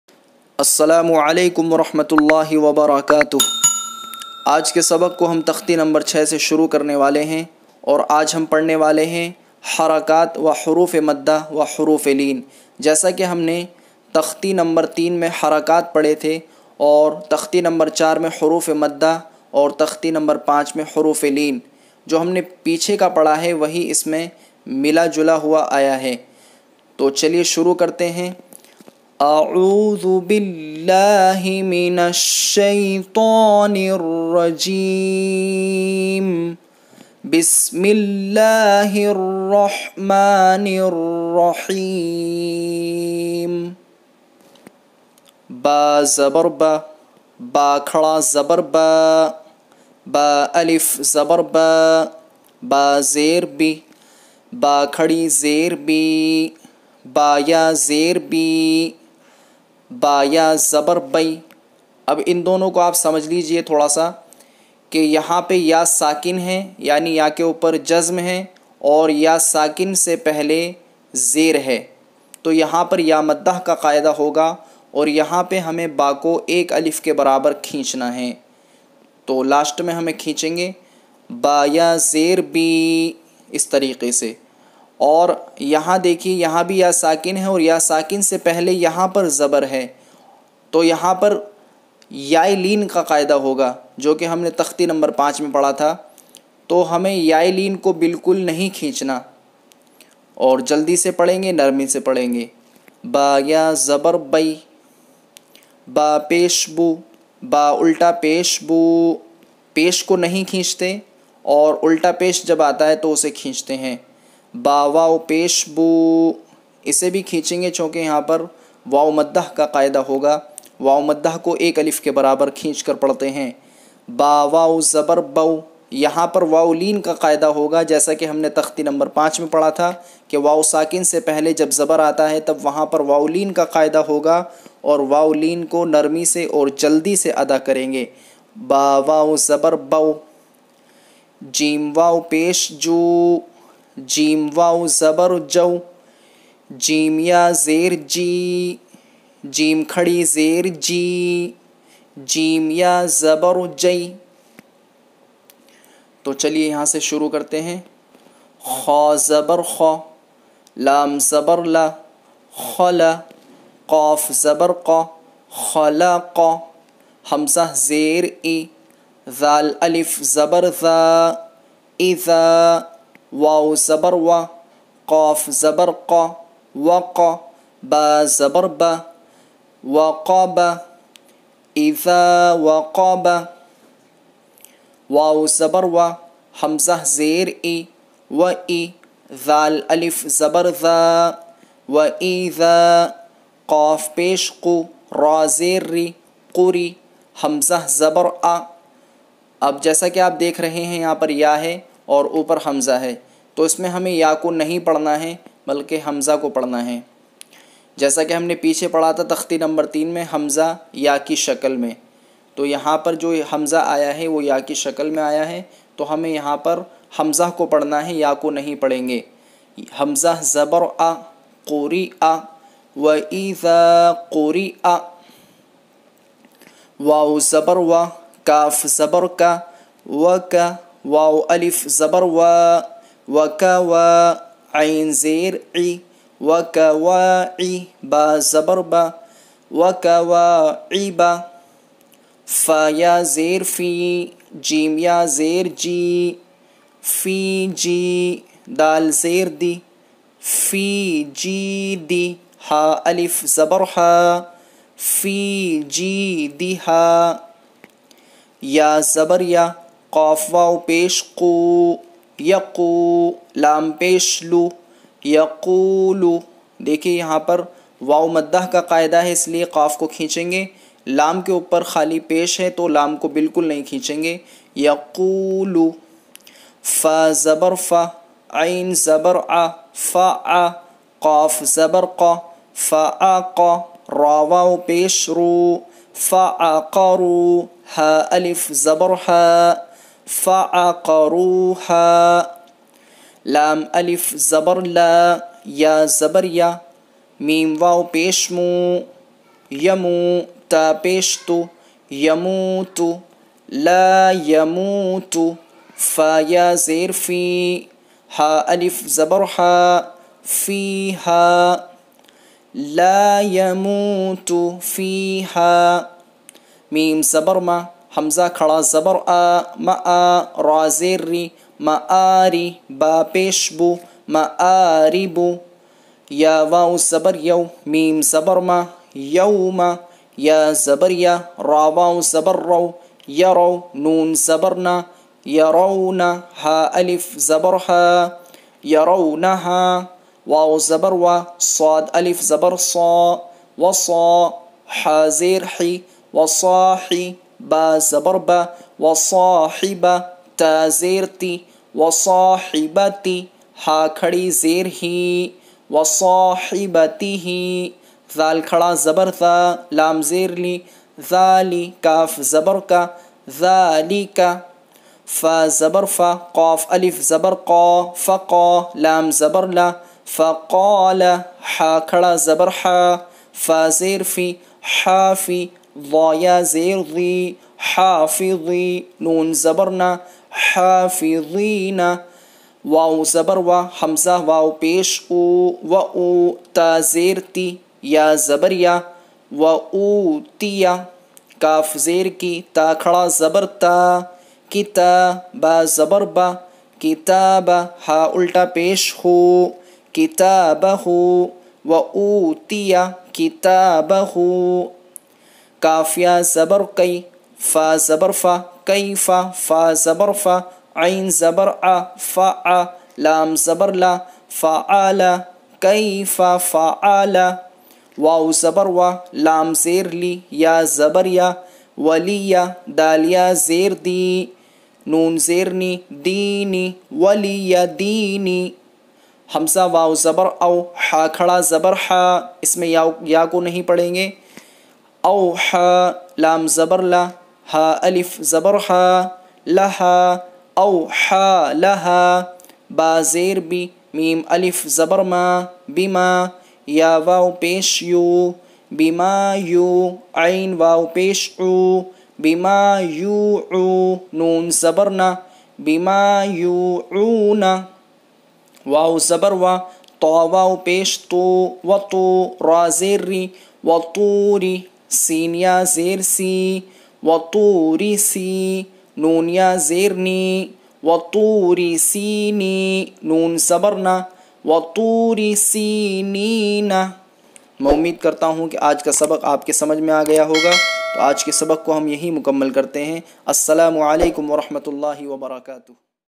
Learn to read the Quraan Part 16 - क़ुरआन पाक पढ़ना सीखें आसानी से | Noorani Qaida
My dear brothers and sisters
Here you will learn Quraan, urdu, sirat, etc
This is a easy way to learn Islam

Aaj ki vidio me hamne padha . Harakaat , Huroofe Maddah, Huroofe Leen ye teenon Qaide bahut eham he Quraane kareem ko Sikhne ke liye.. Is liye me aap se guzaarish kronga aap in Qaidon ko bhut acche se samajh len. Agar ek baar vidio dekhne ke baad samajh na aaye to dobara vidio ko dekhe. Isliye ke ye Qaide basic hen inka jan na bhut zroori he.. Aur hamari koshish he ke aap ko har wo chiz sikhana jis se aap ka quran bhut umdah ho sake.

Allah hamare padhne padhane ko behad aasan farmaye aur jo bhi dushwariyan hen un sabko khatam farmaye Aameen...